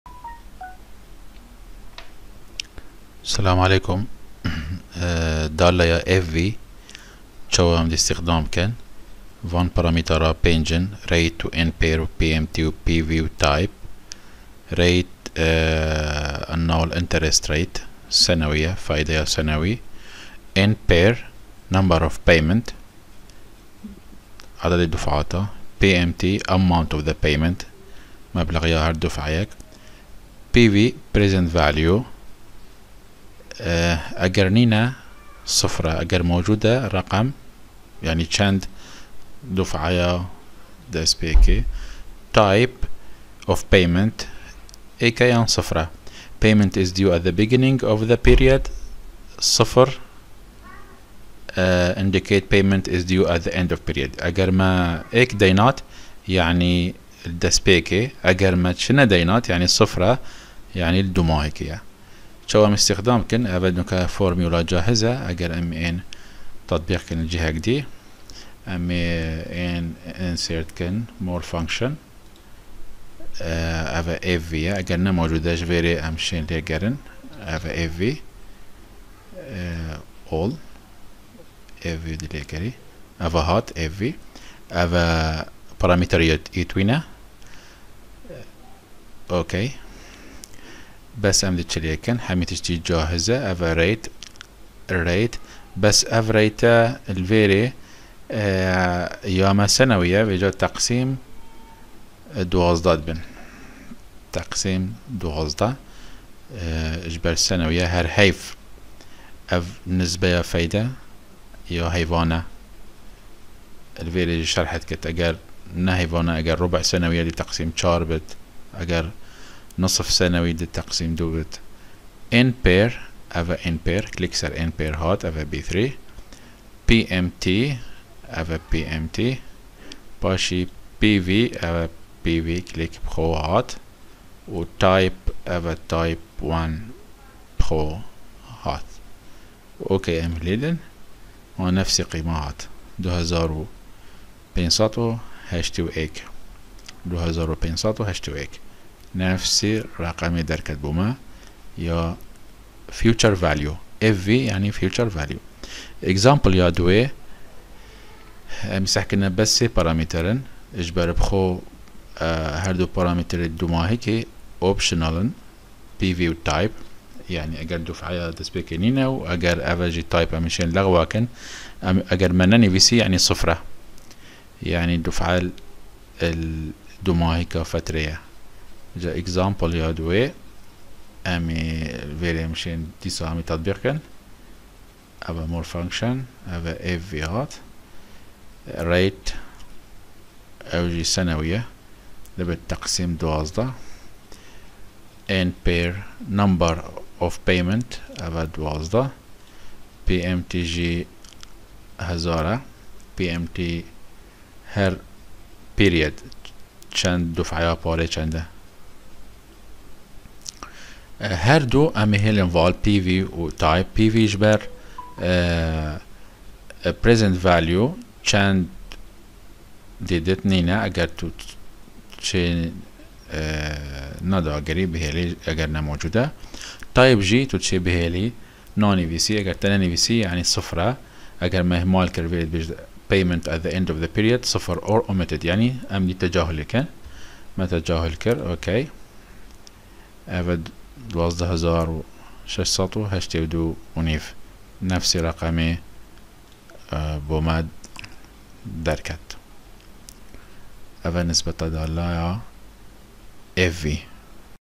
Microsoft Excel Lab Sheet No.7 , FV Function
Microsoft Excel 2016 Lab Sheet No.7
FV Function Explained In Kurdish Language.